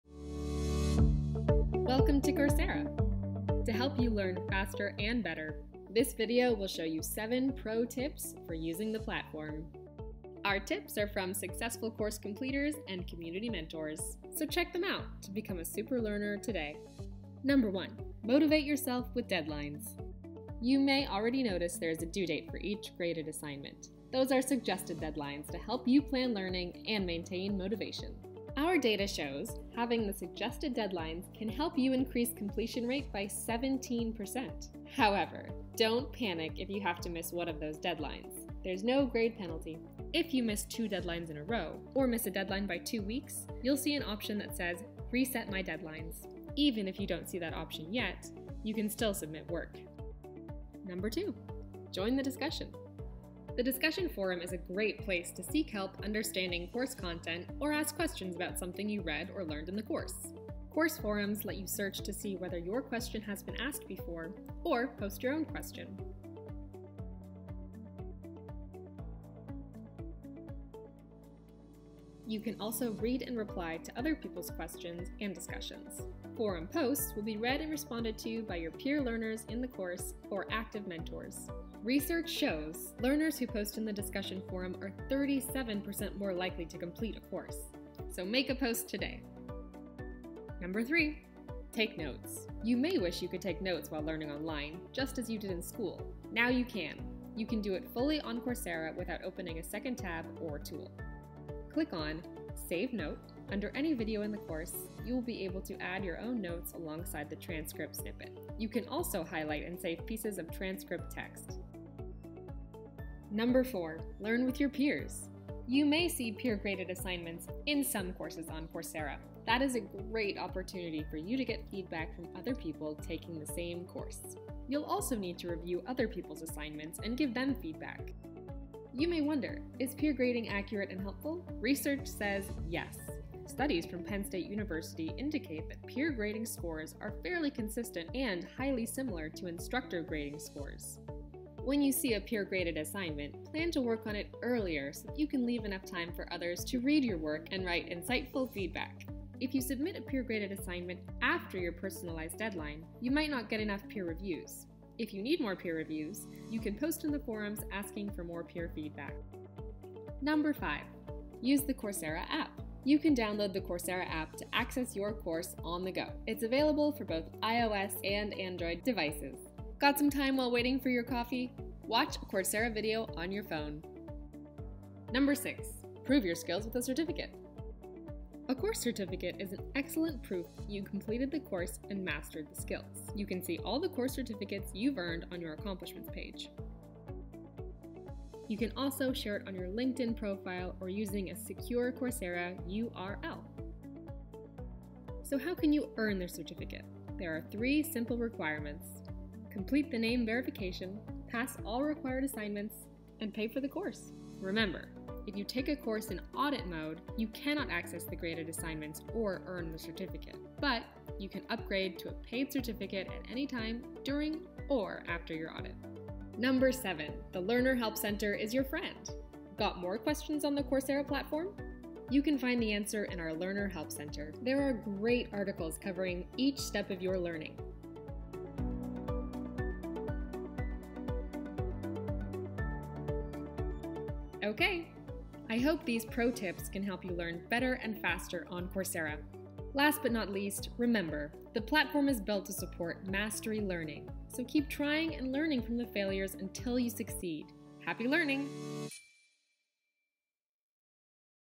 7 Pro Tips for Using Coursera
Taking a Coursera course? Watch this short video for 7 quick tips on how to get the most out of your experience. Connect with Coursera learners in the Coursera Community